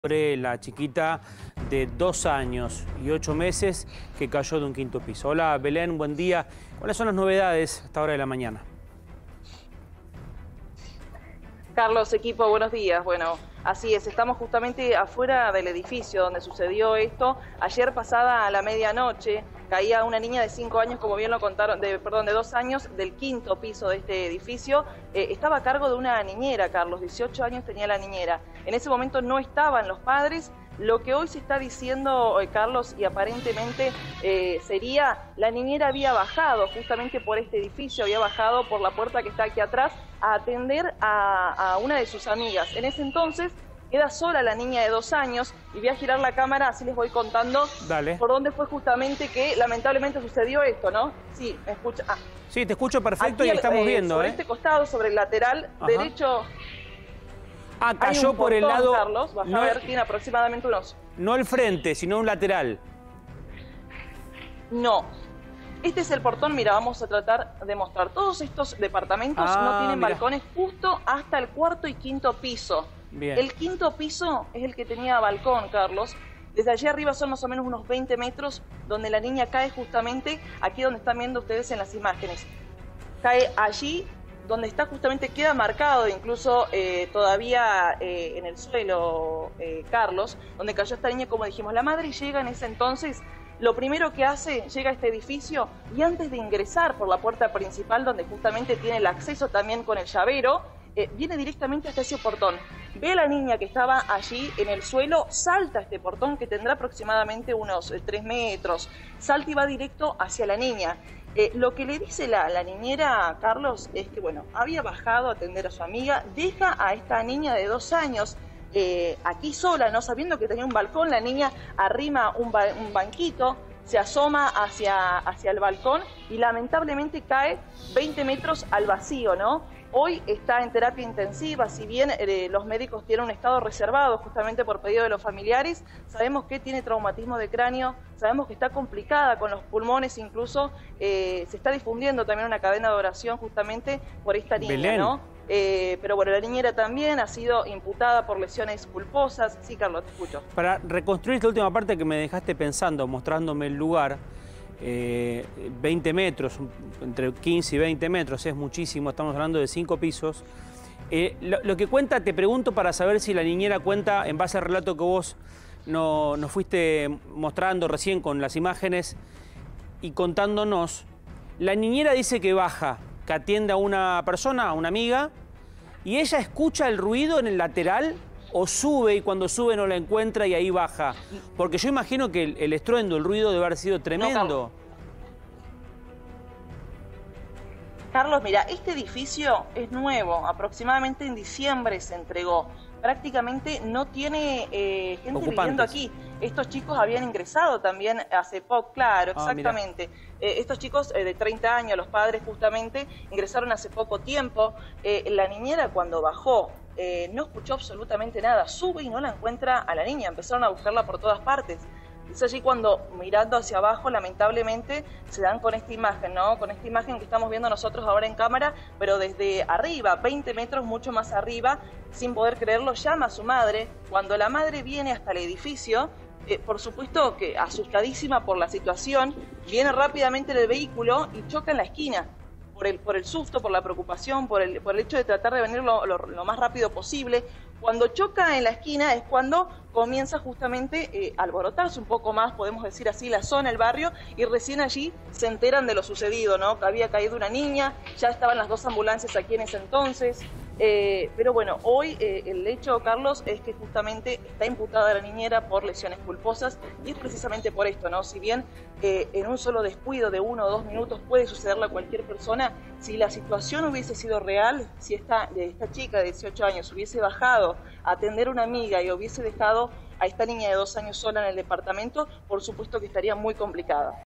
Villa Carlos Paz: una nena cayó de un quinto piso
Fue por un descuido de la niñera, está internada en terapia intensiva.
Belén Bianchi, periodista: "La niñera está imputada por lesiones culposas".